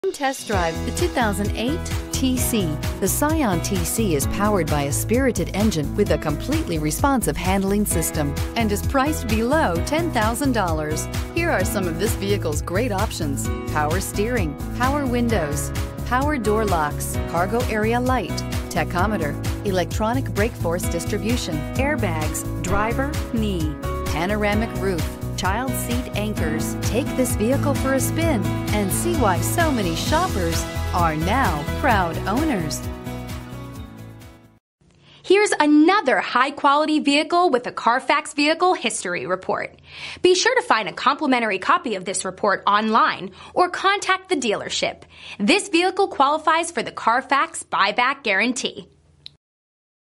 2008 Scion tC S477853A
2008 Scion tC Base

Ewing Subaru
1555 Dallas Parkway

**As-Is**Fresh New Car Trade**Auction Bound** Has Hail Damage**. This vehicle will be sold as-is before it goes to auction. The vehicle will get a detail inside and out also a safety inspection and oil change. We welcome pre-buy inspections with a mechanic of your choice.